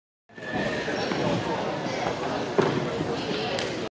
Péceli Tamás (2004) — magasugrás 1.66 m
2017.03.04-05. Újonc és Serdülő Fedettpályás Összetett Bajnokság